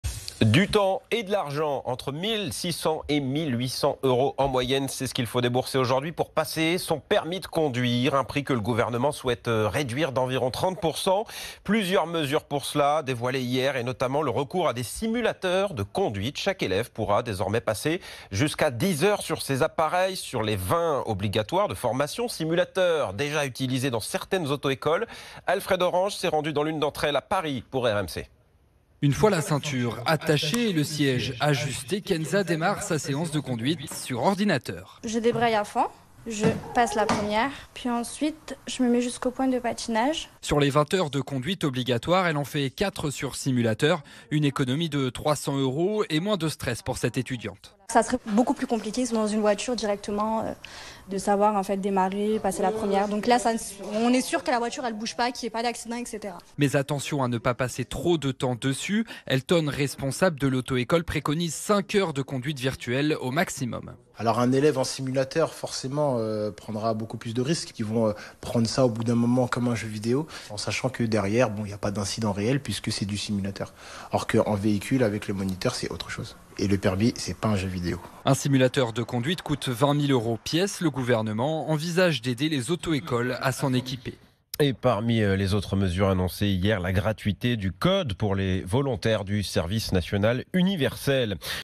Pour un permis de conduire moins cher, le gouvernement veut encourager à la formation sur simulateur
Le gouvernement veut réduire le prix du permis de 30%. Pour cela, il va autoriser jusqu'à 10 heures d'apprentissage sur simulateur contre seulement 5 actuellement.